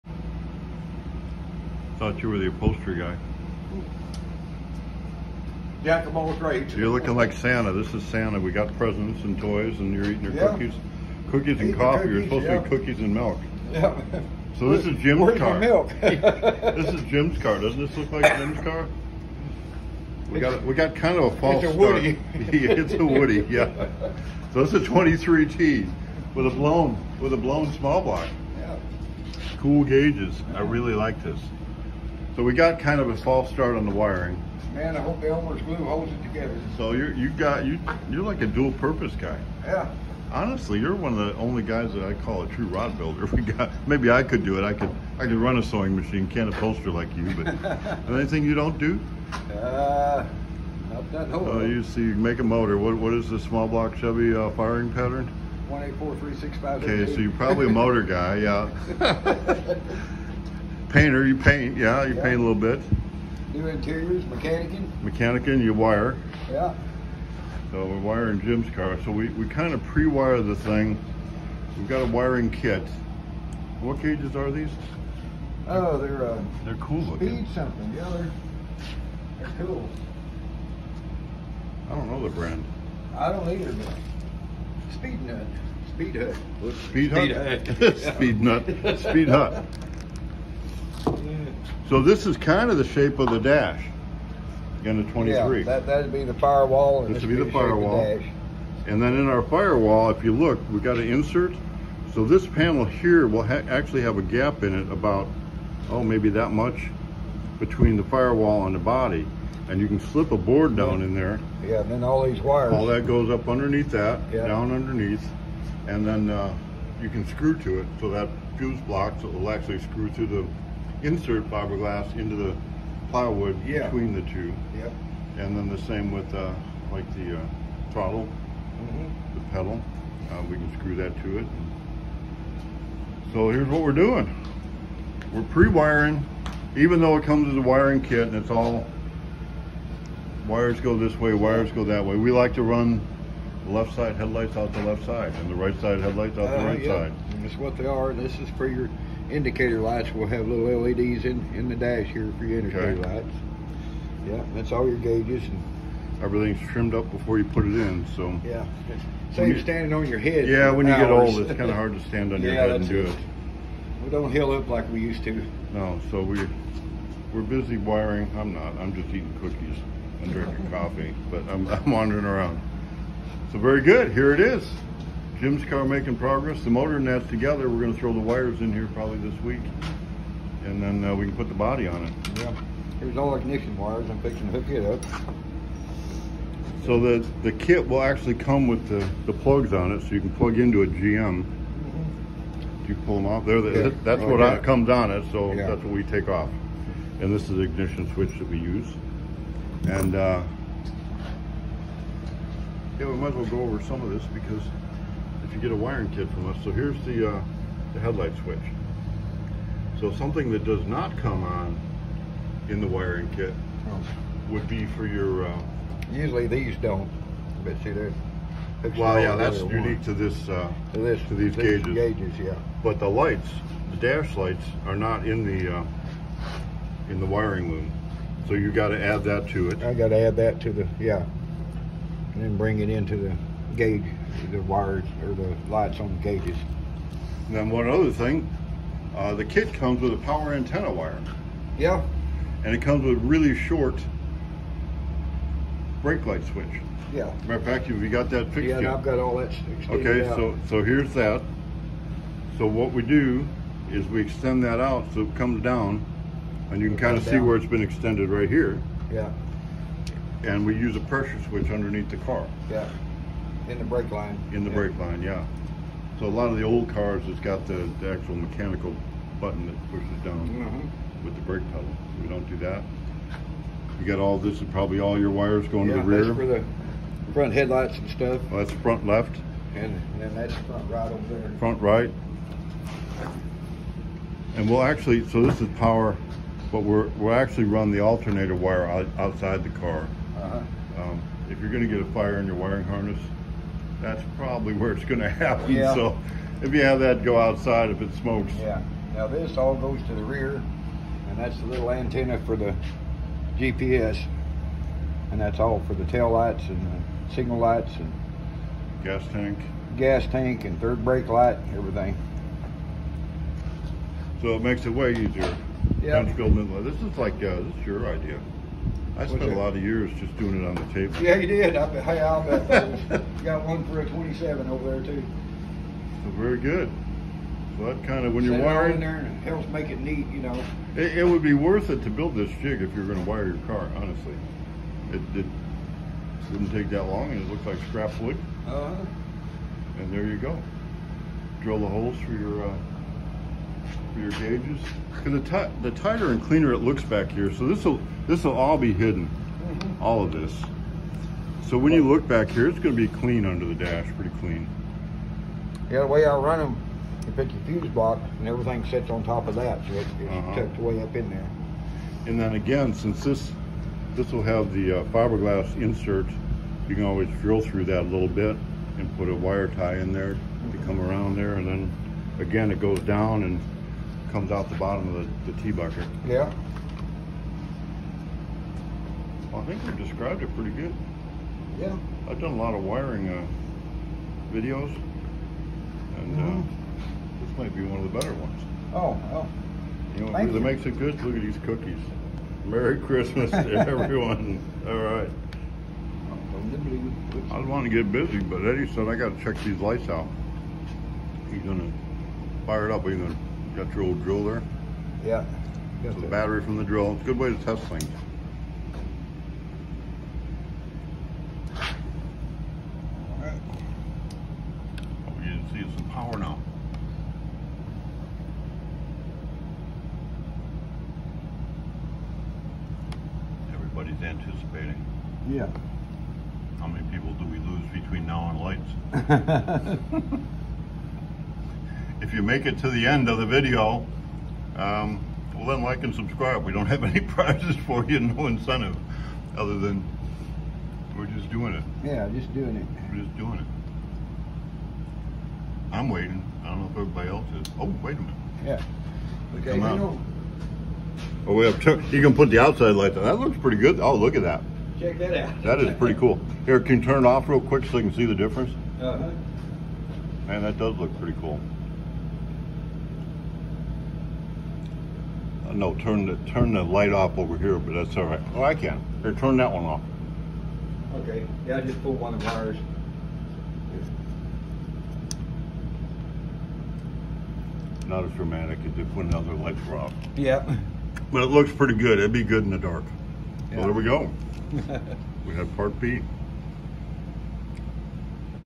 Wiring a 1923 T bucket
Building the wire loom for Jim’s 1924 blown T bucket